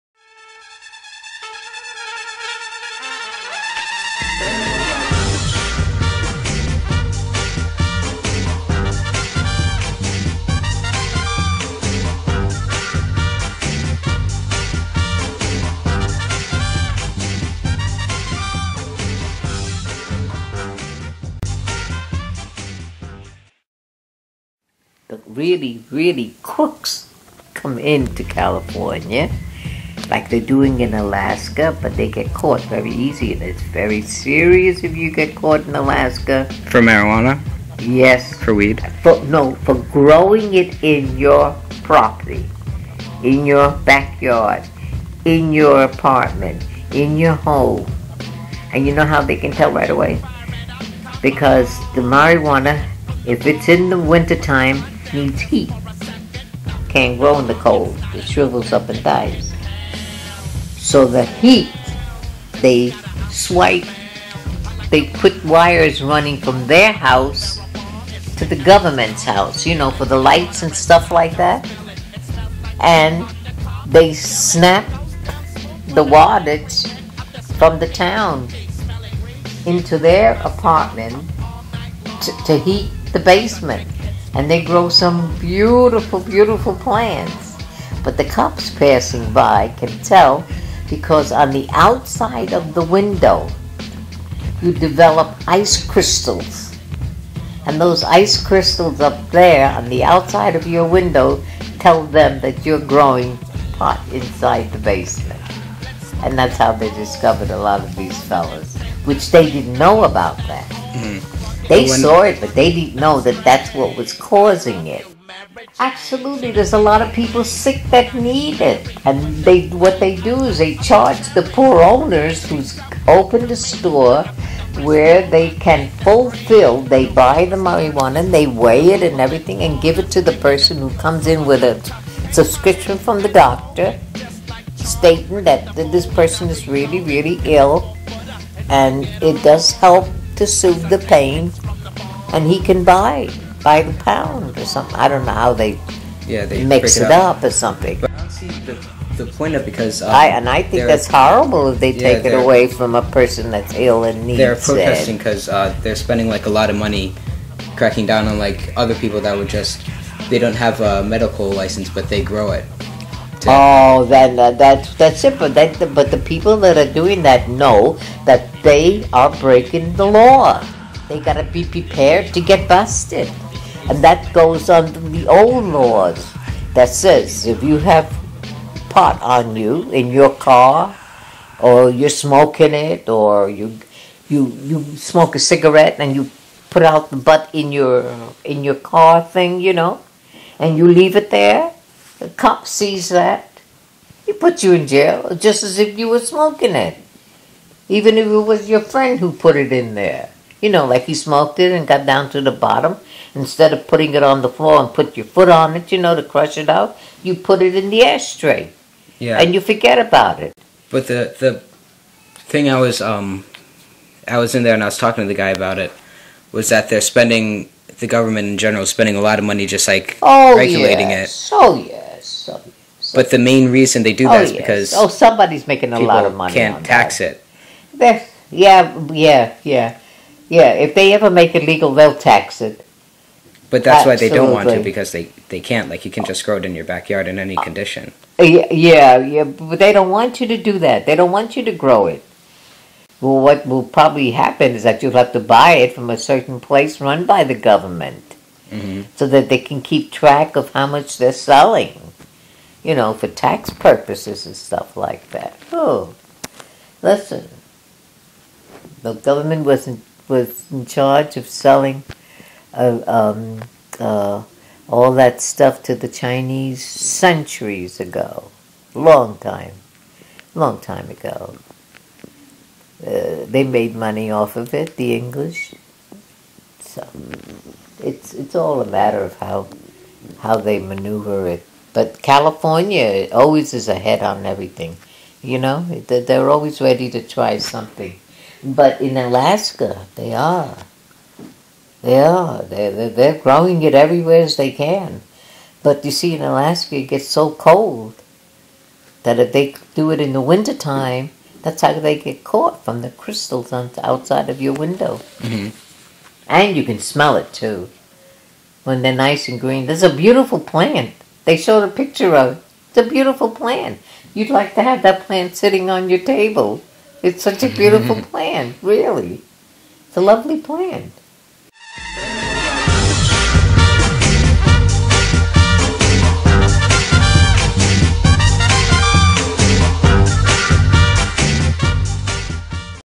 SH*T NANA SAYS (PT 1. Nanna KUSH!)
In this very first episode of "Shit Nana Says" we cover the history of weed with my grandmother. Sit back, and educate YO SELF FOOS!